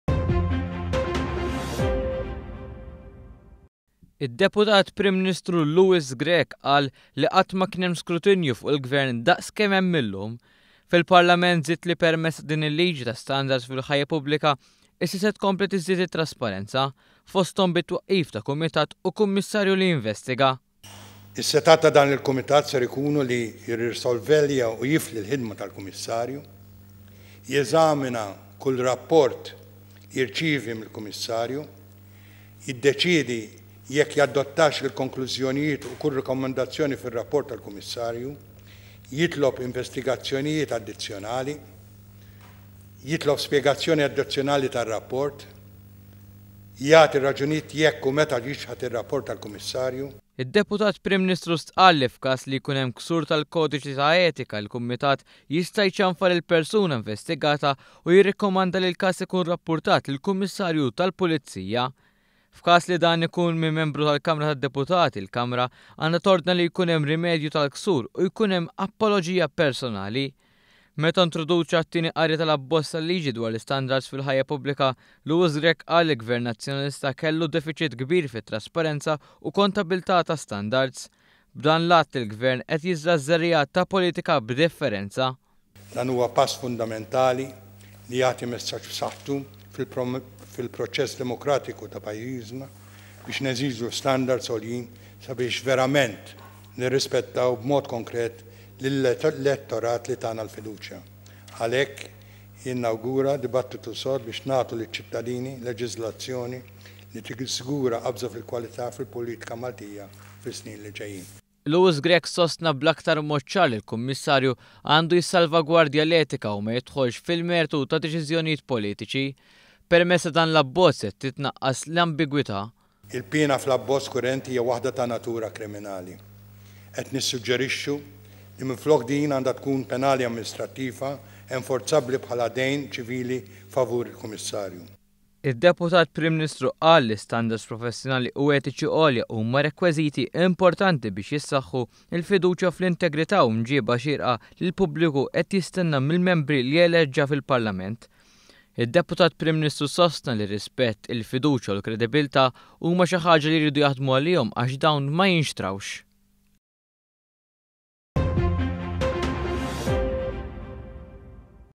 AKTAR TRASPARENZA BIL-LIĠI TAL-ISTANDARDS.
ONE.COM.MT • It-tieni qari tal-abozz dwar l-standards fil-ħajja pubblika qed jkompli juri kemm Gvern Laburista huwa kommess li jibni fiduċja u trasparenza fil-politika Maltija. Id-deputat Prim Ministru Louis Grech fil-Parlament ta' spejgazzjoni ta’ diversi emendi li qed isiru sabiex dan ikun possibli.